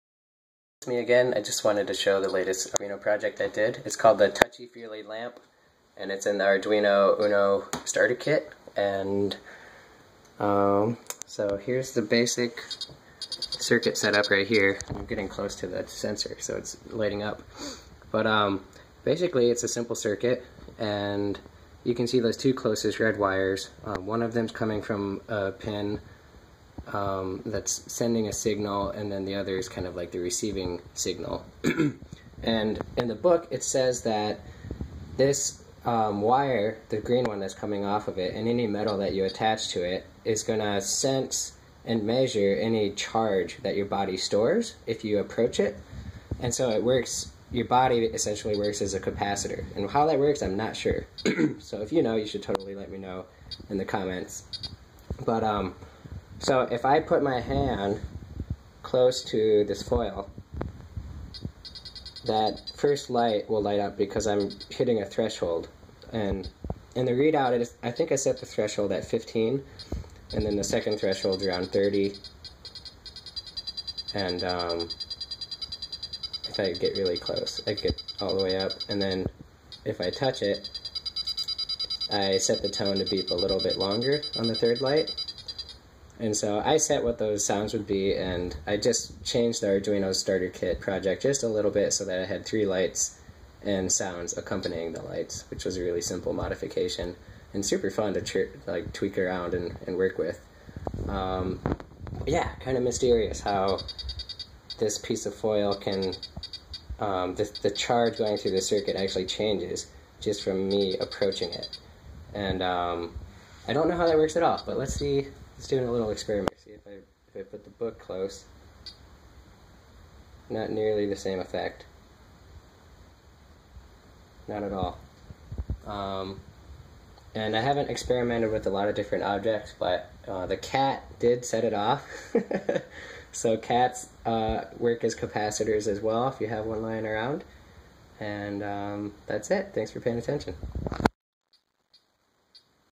arduino touch sensitive light
This is also pretty much straight from the arduino starter kit except I added 2 more lights, 3 more thresholds, and a piezo.

The sketch took a bit of experimentation and tweaking but I really like how it turned out!